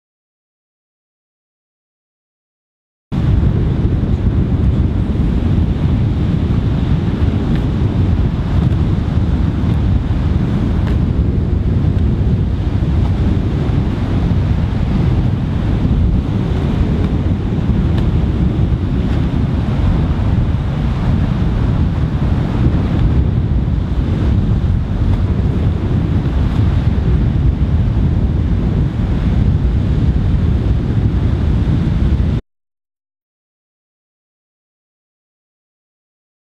Gekisai dai ichi Kata Karate Goju
Karate goju ryu Kata
Kata gekisai dai ichi
Filmed at 10-May-2016
Preformed by : Eli Elfassy

Camera Canon 700d lens 50mm  f 1.8